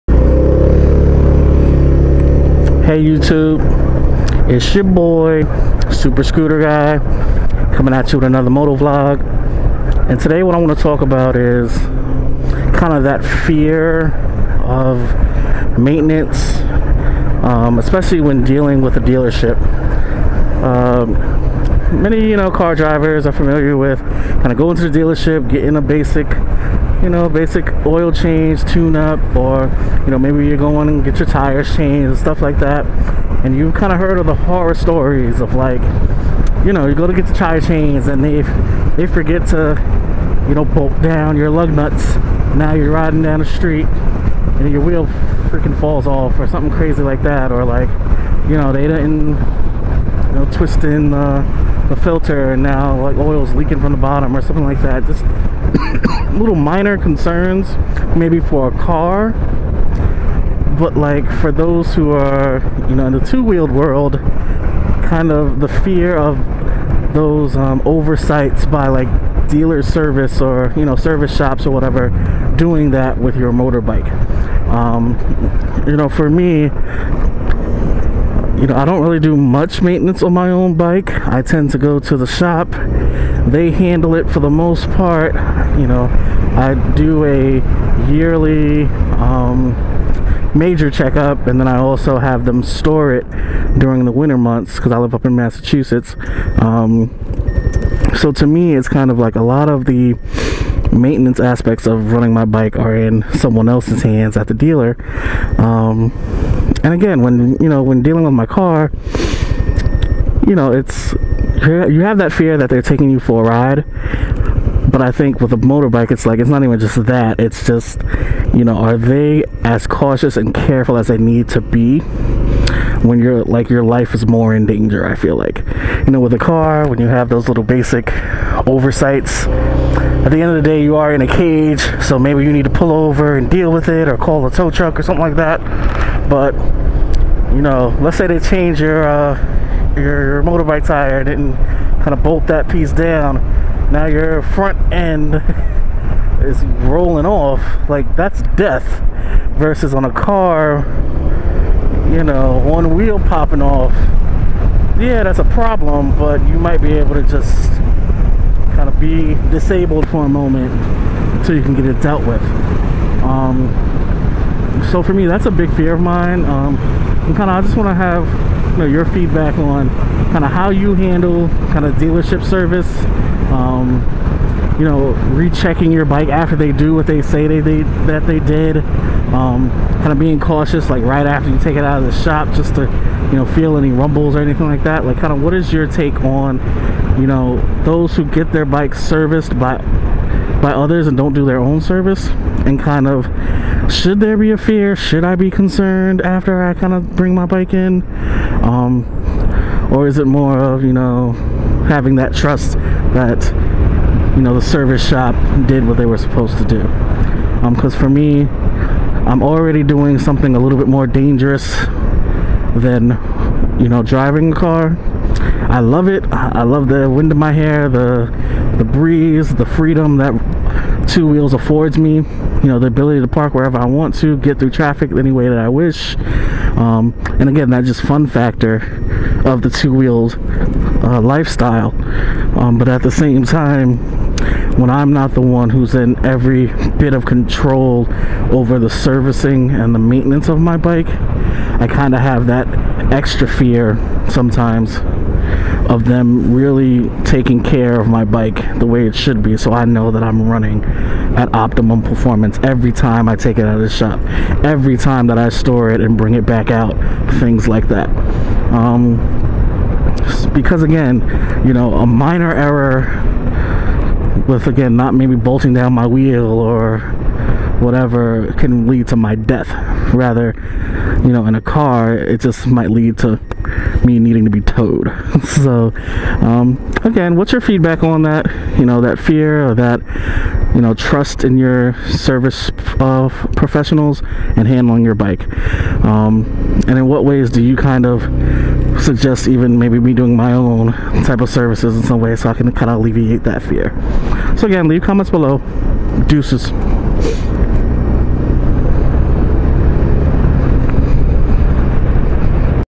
Moto Vlog #35 - Service Shop Fears
In this moto vlog I discuss my concerns with servicing my bike at the shop versus the pitfalls of servicing a car at a service shop.